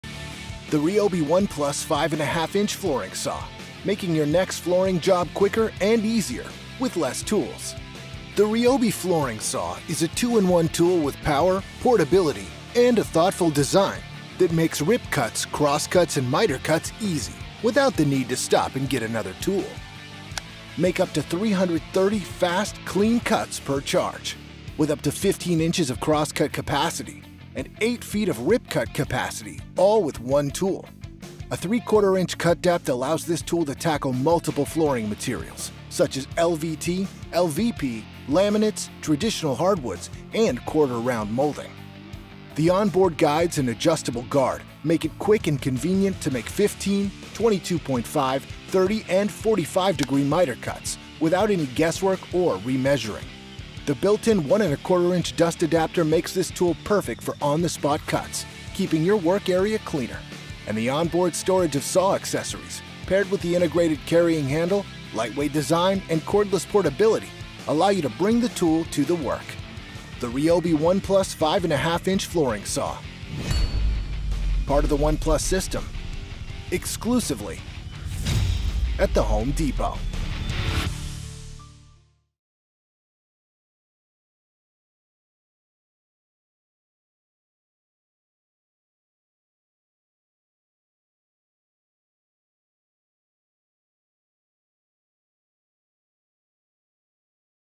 NEW! | 18V ONE+ 5-1/2" Flooring Saw
With no outlets or extension cords needed, this 18V ONE+ 5-1/2" Flooring Saw (PGC21) saw helps you work faster and more efficiently no matter where the job takes you!